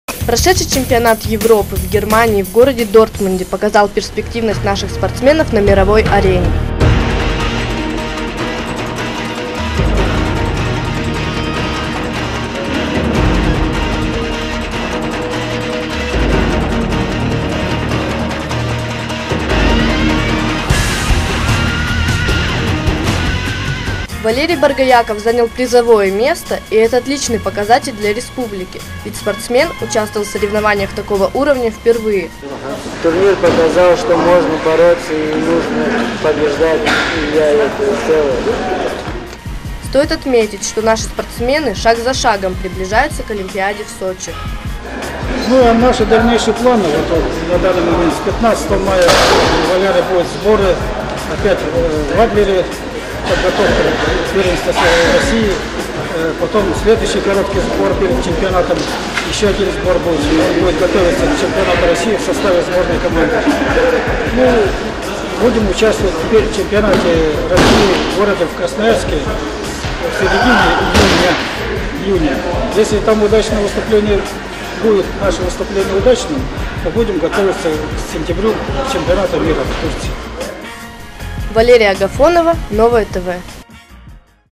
Хакасии нужны победы! Мы в нее верим!
Прошедший чемпионат Европы в Германии в городе Дортмунде показал перспективность наших спортсменов на мировой арене.

Валерий Боргояков занял  призовое место и это отличный показатель для республики, ведь спортсмен участвовал в соревнованиях такого уровня впервые. Стоит отметить, что наши спортсмены шаг за шагом приближаются к олимпиаде в Сочи.